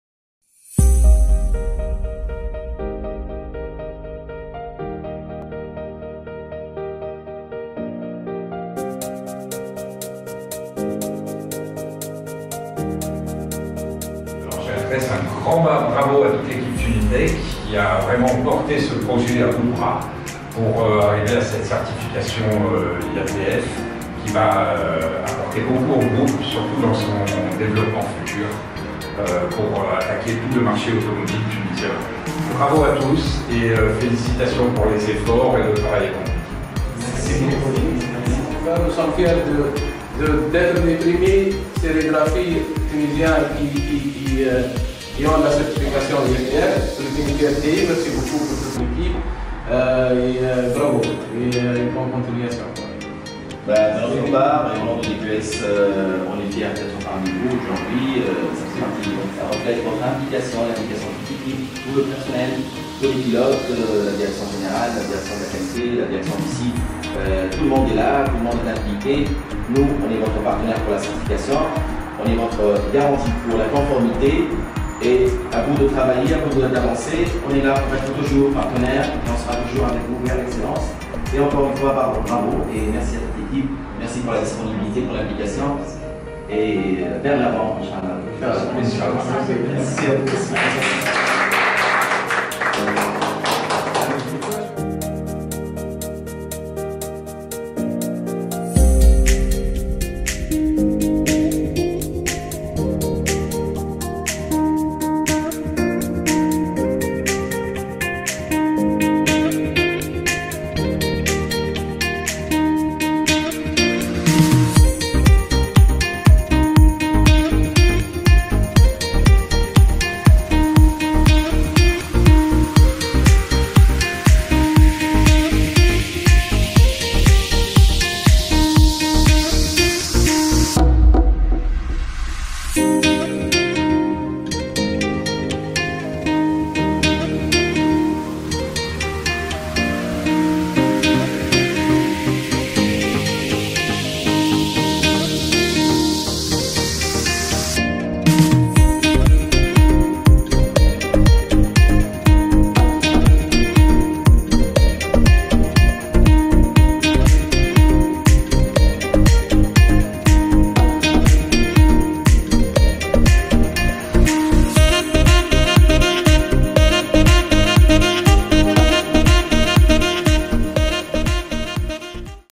Tunitec Labels : Certification IATF 16949
Alliant technologie et savoir-faire, Tunitec Labels est devenue le partenaire stratégique des plus grandes marque dans le domaine du marquage industriel, de l'impression, et de la découpe technique... Certifiée IATF 16949, elle assure un contrôle de qualité qui frôle la perfection ! 💯 💯
Toutes nos félicitations à l'équipe Tunitec, et à leur tête, M. Zine Dgachi et M. Anis Othmani ! 👏🏽👏🏽